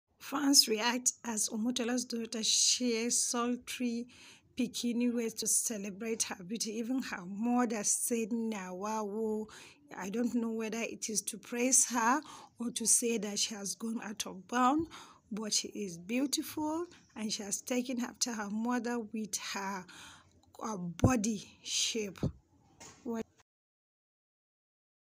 Omotola Jakande's Daughter Shares Hot Bikini Pictures
Omotola Jakinde daughter decided to wear bikini wear to show the world her beautiful bodily features. Although some have argued that it was enhanced, it can be said that she took after her mother. The picture brought alot of negative and positive responses.
Remember, the African society is a traditional society and doesn't doesn't console nudity. Infact even mother commented "Na Wa O", showing her surprise. We await to see what the beautiful model has in store for us for the future.